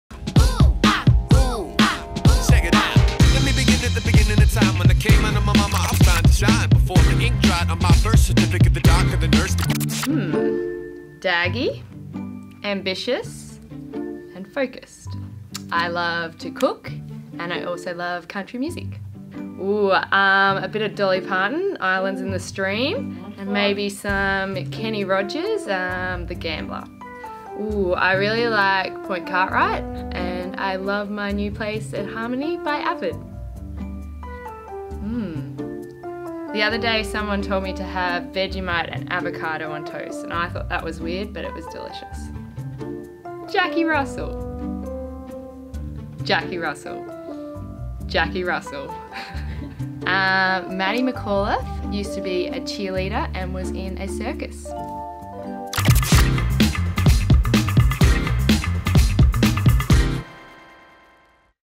Get to Know: LAURA SCHERIAN
Get To Know Sunshine Coast Lightning player Laura Scherian thanks to NCP.